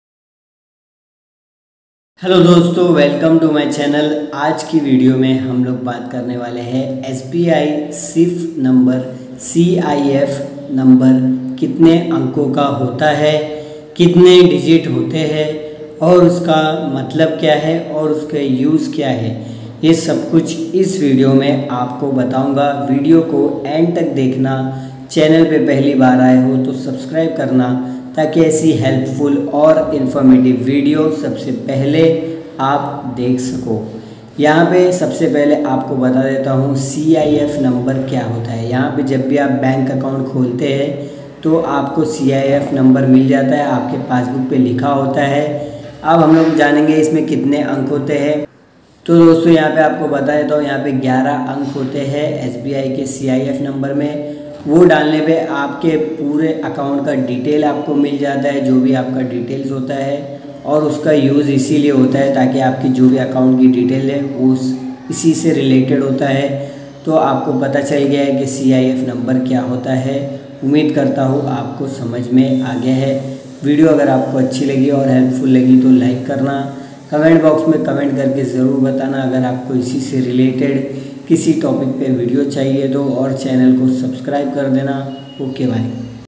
sbi cif number kitne digit ka hota hai | how many digits in sbi cif number
00:00 sbi cif number kitne digit ka hota hai
00:30 how many digits in sbi cif number
01:30 how many digits in cif number
02:00

in this video I'm talking about sbi cif number kitne digit ka hota hai